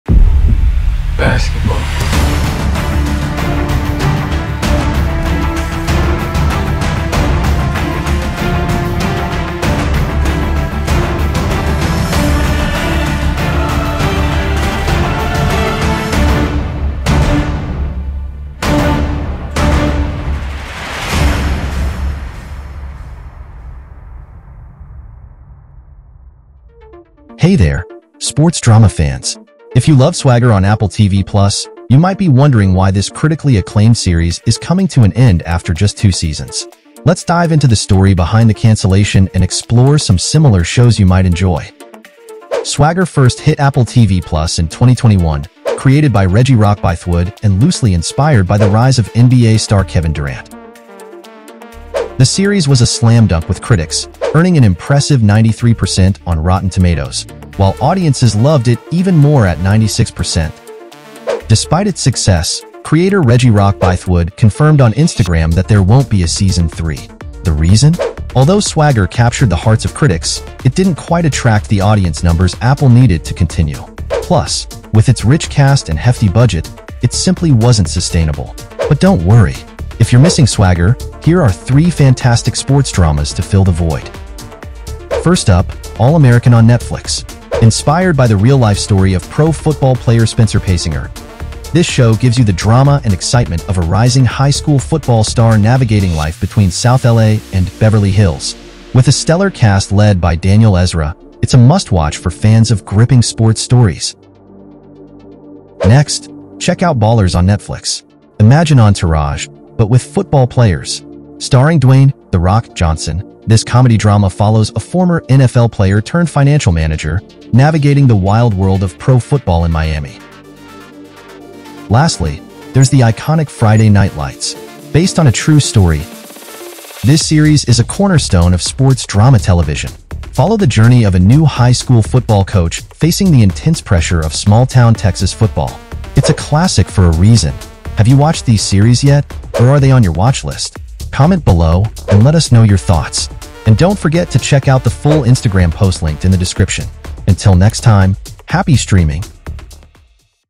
Swagger Season 3 PREVIEW | Apple TV+ | Is it really Canceled?
This is a Swagger Season 3 PREVIEW, Learn every detail about why and how Swagger Season 3 has been canceled by Apple TV+Follow. In this video, we have also discussed a few of the recommendations of Sports Drama like Swagger.

Watch the video till the end to know about every details related to Swagger Season 3.